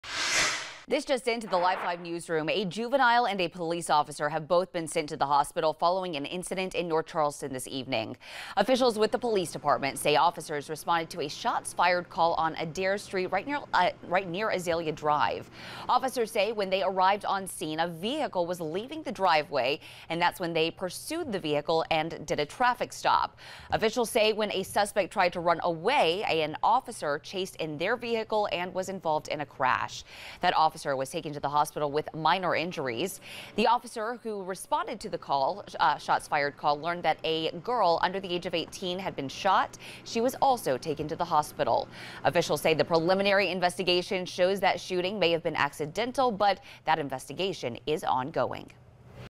VIDEO: Juvenile shot, N. Charleston officer injured in crash
North Charleston Police say one of their officers was hurt Thursday night in a pursuit after responding to a reported shooting involving an underage girl.